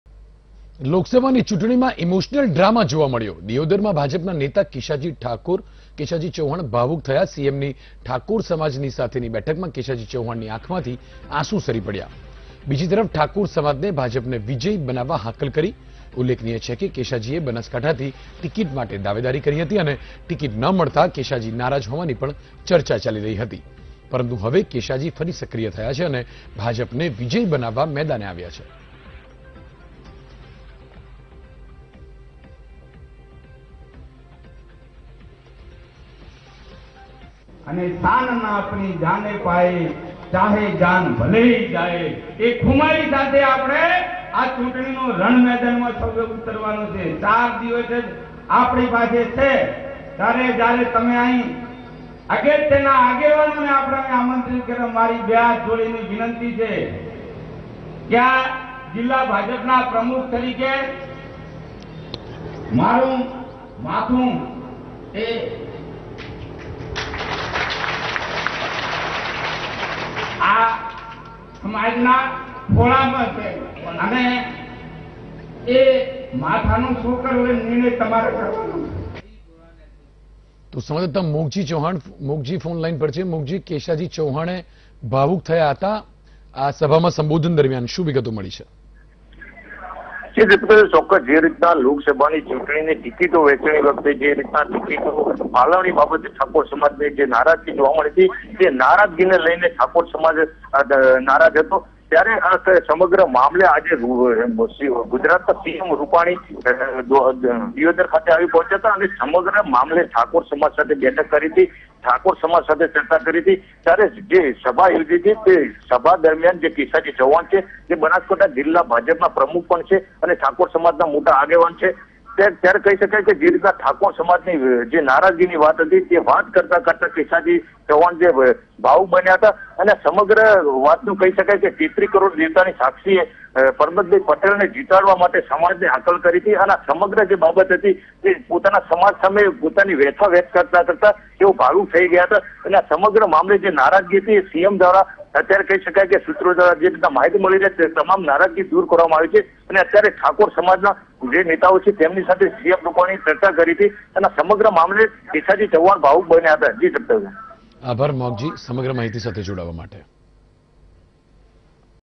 LokSabha ના જંગમાં ઈમોશનલ ડ્રામા, Gujarat ના આ નેતા બેઠકમાં રડી પડ્યાં | Vtv Gujarati
LokSabha ના જંગમાં ઈમોશનલ ડ્રામા, Gujarat ના આ નેતા બેઠકમાં રડી પડ્યાં.